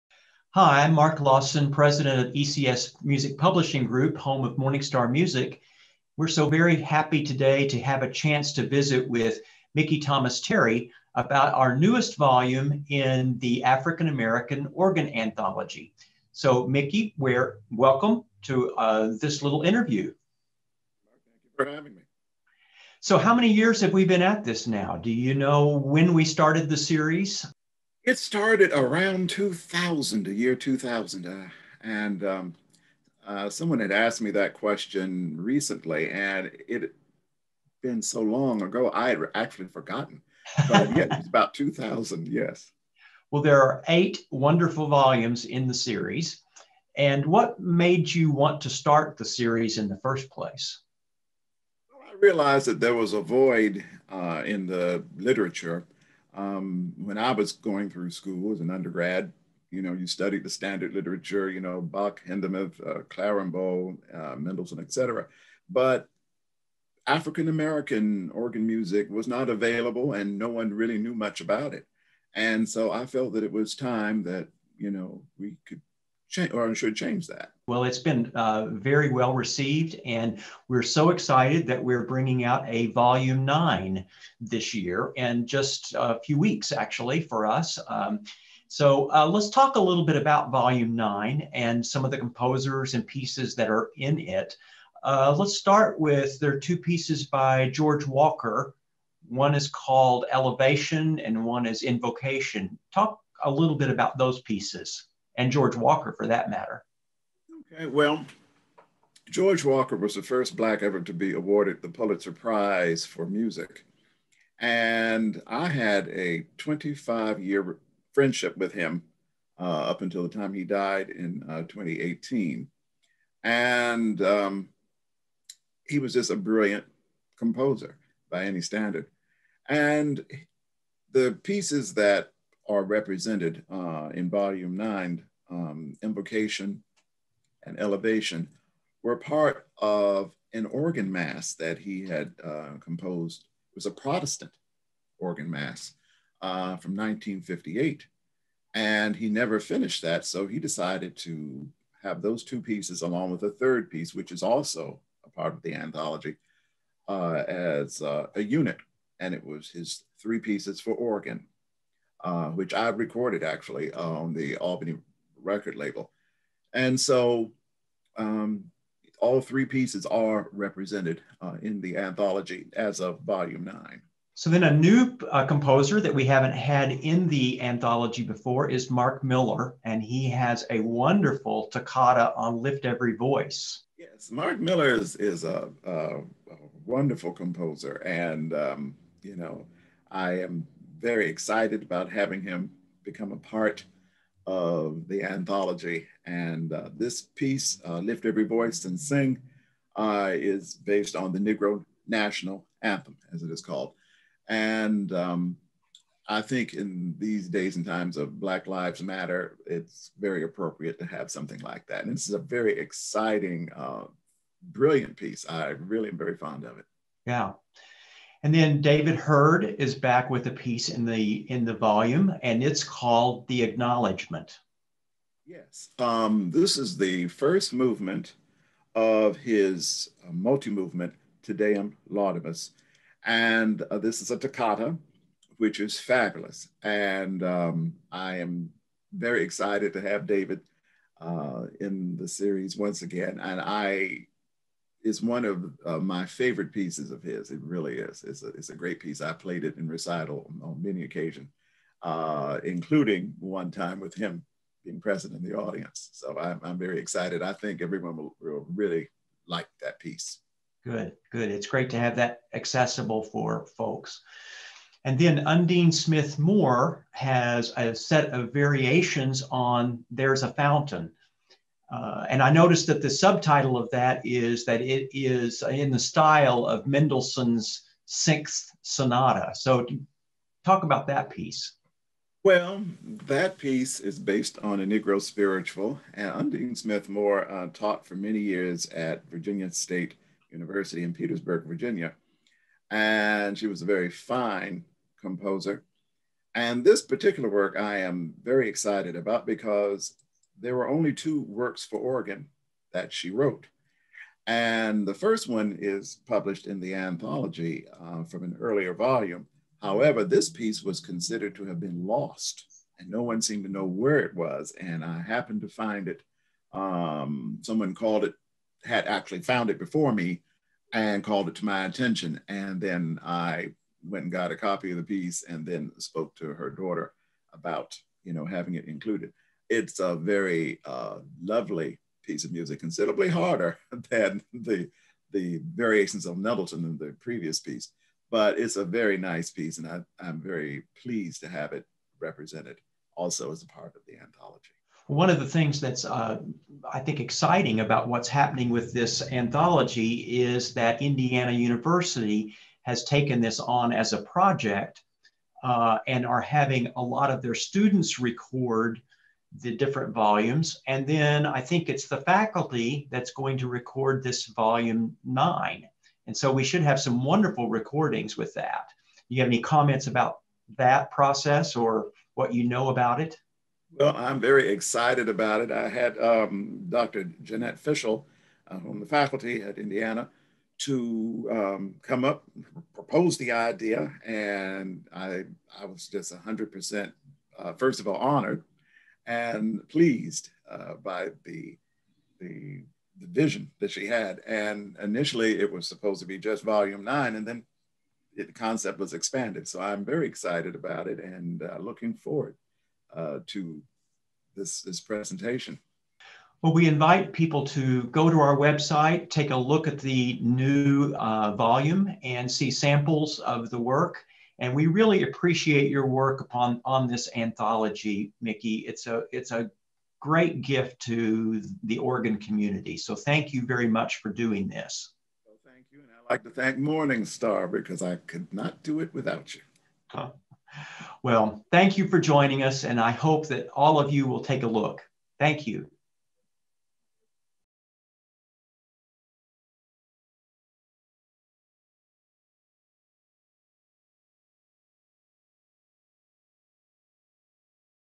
Mickey Thomas Terry on the NEW Volume of the African-American Organ Music Anthology (Vol. 9)
Dr. Mickey Thomas Terry joins Mark Lawson in conversation about the new volume in the African-American Organ Music Anthology. They discuss the history of the series, some of the new pieces in Vol. 9, and about an upcoming project by Indiana University to record all nine volumes!